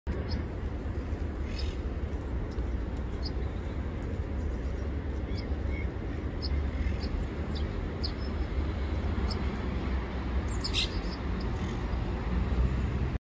El Juego del Carbonero...
Carbonero Común (Parus major).
Imágenes tomadas el 17 de Diciembre del 2021 por la ladera del río Manzanares, Madrid.

Sonido ambiente.